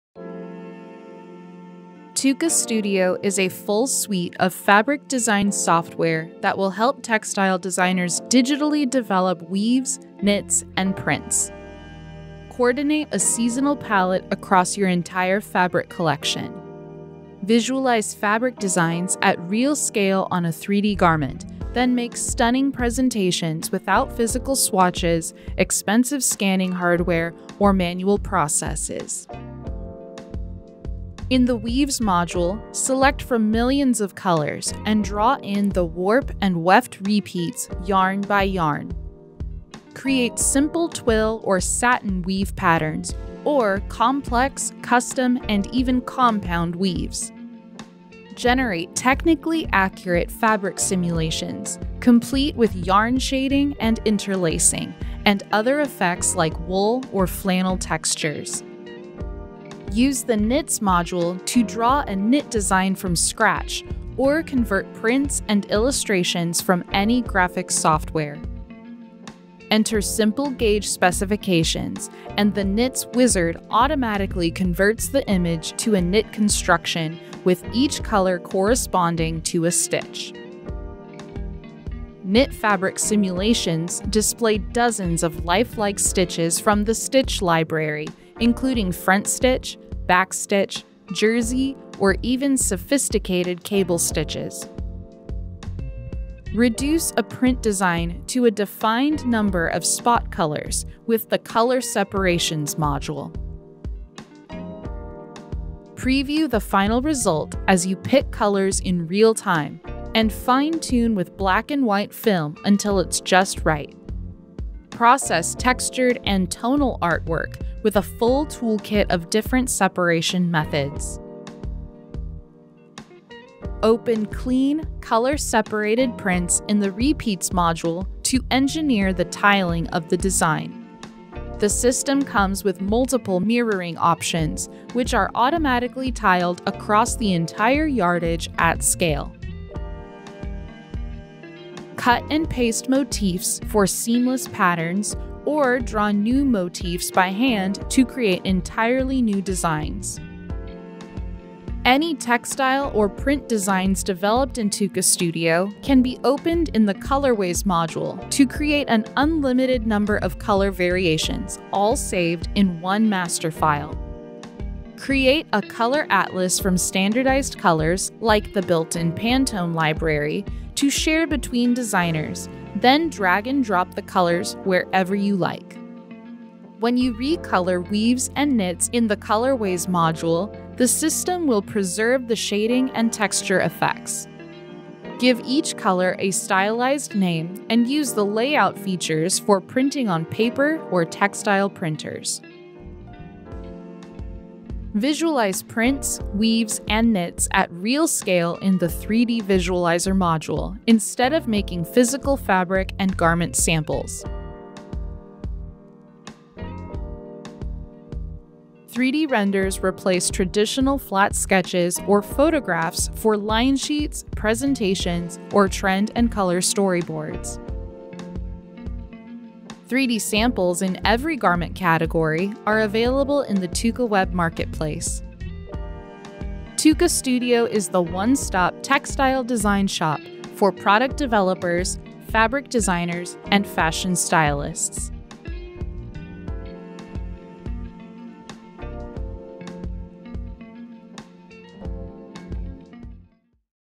Introduction to TUKAstudio | One Stop Textile Design Shop | Fabric Design Software with 3D
TUKAstudio is the one-stop textile design shop for product developers, fabric designers, and fashion stylists. Visualize fabric designs at real scale on a 3D garment, then make stunning presentations without physical swatches, expensive scanning hardware, or manual processes.

OUTLINE:
00:00 - Introduction
00:29 - Weaves
00:58 - Knits
01:35 - Color Separation
02:02 - Repeats
02:31 - Colorways
03:13 - 3D Visualizer